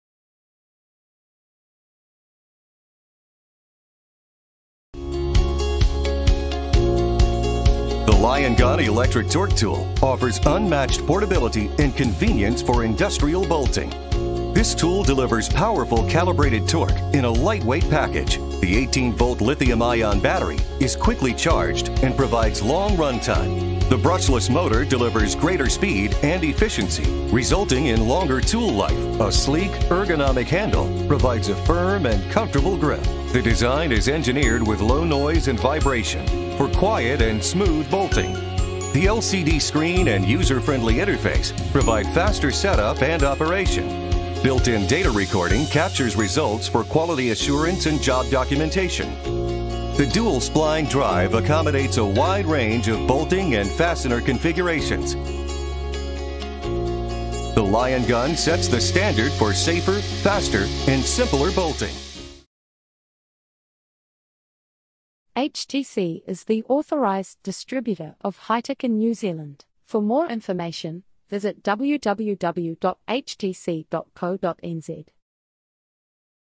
LION GUN ELECTRIC TORQUE WRENCH
The LION GUN Electric Torque Tool with built-in data recording offers unmatched portability and convenience, simplifying safe and accurate bolting. This 18-Volt battery powered tool has a torque capacity of up to 700 ft-lbs. and the capability to record jobs performed. This tool is compatible with conventional sockets, the HYTORC Washer, and the HYTORC Nut.

HTC is the authorised distributor of Hytorc in New Zealand.